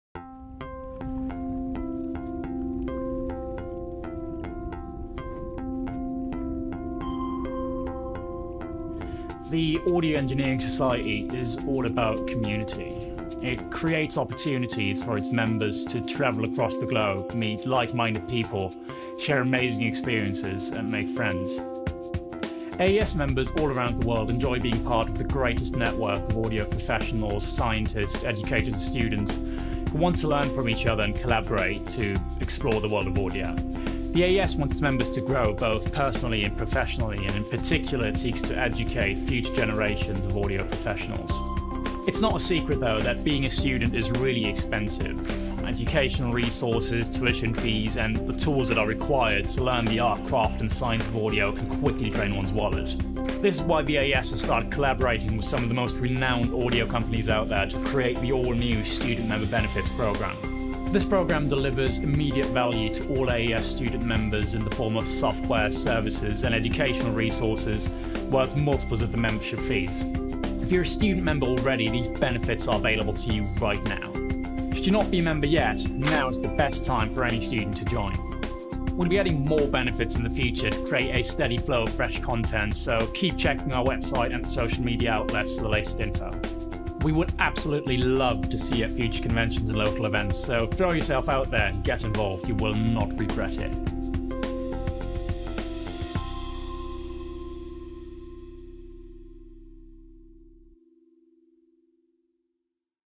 AES Student Member Benefits Program
Audio Engineering Society Student Members receive a number of freebies and exclusive deals from world renowned audio companies. Be sure to make use of the exciting benefits offered by our partners Video created by Simon-Claudius Wystrach